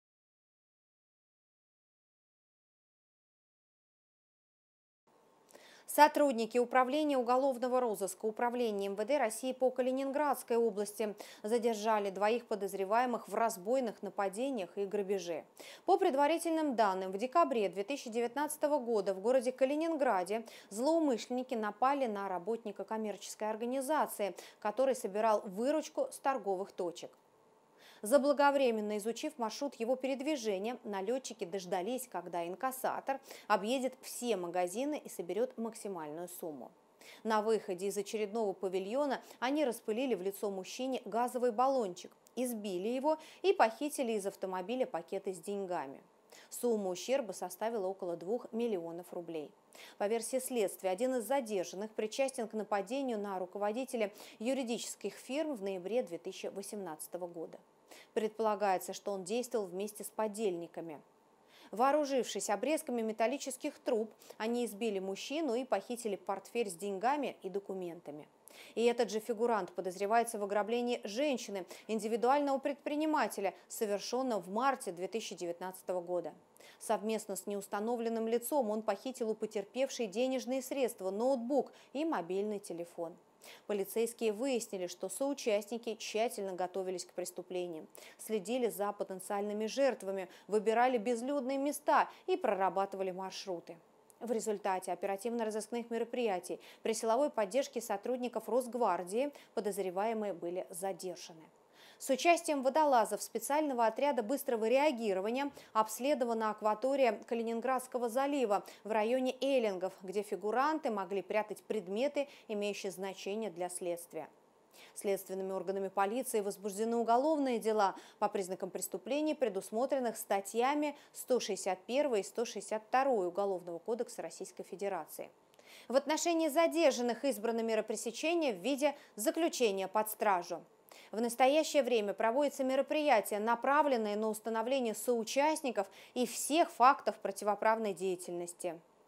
Полицейские Калининграда задержали подозреваемых в совершении преступлений против собственности
Сотрудники Управления уголовного розыска Управления МВД России по Калининградской области задержали двоих подозреваемых в разбойных нападениях и грабеже.

По предварительным данным, в декабре 2019 года в городе Калининграде злоумышленники напали на работника коммерческой организации, который собирал выручку с торговых точек. Заблаговременно изучив маршрут его передвижения, налетчики дождались, когда инкассатор объедет все магазины и соберет максимальную сумму. На выходе из очередного павильона они распылили в лицо мужчине газовый баллончик, избили его и похитили из автомобиля пакеты с деньгами. Сумма ущерба составила около 2 млн рублей.

По версии следствия, один из задержанных причастен к нападению на руководителя юридических фирм в ноябре 2018 года. Предполагается, что он действовал вместе с подельниками. Вооружившись обрезками металлических труб, они избили мужчину и похитили портфель с деньгами и документами.

Этот же фигурант подозревается в ограблении женщины, индивидуального предпринимателя, совершенном в марте 2019 года. Совместно с неустановленным лицом он похитил у потерпевшей денежные средства, ноутбук и мобильный телефон.

Полицейские выяснили, что соучастники тщательно готовились к преступлениям: следили за потенциальными жертвами, выбирали безлюдные места и прорабатывали маршруты.

В результате оперативно-розыскных мероприятий при силовой поддержке сотрудников Росгвардии подозреваемые задержаны.

С участием водолазов специального отряда быстрого реагирования обследована акватория Калининградского залива в районе Эллингов, где фигуранты могли прятать предметы, имеющие значение для следствия.

Следователями отделов МВД России по Центральному и Ленинградскому районам города Калининграда возбуждены уголовные дела по признакам преступлений, предусмотренных статьями 161 и 162 Уголовного кодекса Российской Федерации. В отношении задержанных, 59-летнего калининградца и его 55-летнего подельника, избрана мера пресечения в виде заключения под стражу.

В настоящее время проводятся мероприятия, направленные на установление соучастников и всех фактов противоправной деятельности.